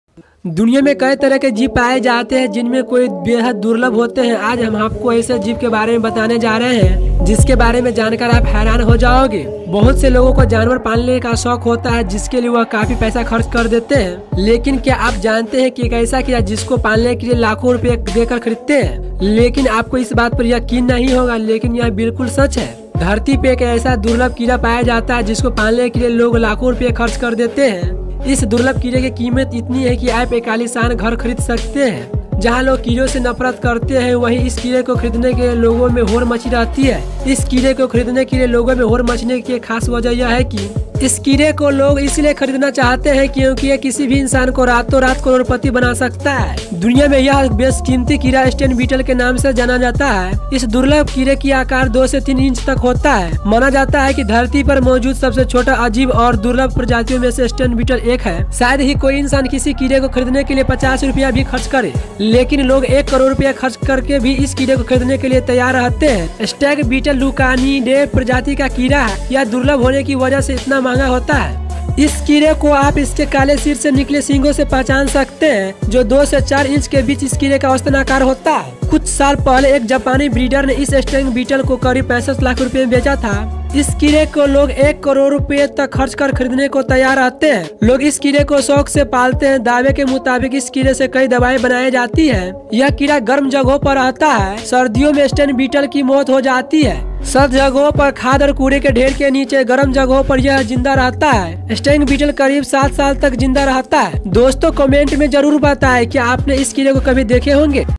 दुनिया के सबसे महंगा कीड़ा/most expensive insects in the world#fact#insects#hindi fact#amazing#viral
दुनिया के सबसे महंगा कीड़ा
/most expensive insects in the world
hindi fact
Stag Beetle
facts video
amazing video

इस धरती पर कई प्रकार के कीड़े पाए जाते हैं। जिनमें से कई तो काफी खतरनाक होते हैं। लेकिन हम बताने वाले दुनिया के सबसे महंगे कीड़े के बारे में।जिंनका कीमत सुनकर आप हैरान हो जाओगे। इसका उपयोग कई तरह के दावा बनाने में किया जाता है। स्टैग बीटल को दुनिया का सबसे महंगा कीड़ा माना जाता है। कुछ साल पहले एक जापानी ब्रीडर ने लाखो रूपाये में खरीदे थे। स्टैग बीटल पृथ्वी पर मौजूद सबसे छोटे, अजीब और दुर्लभ प्रजातियों में से एक है।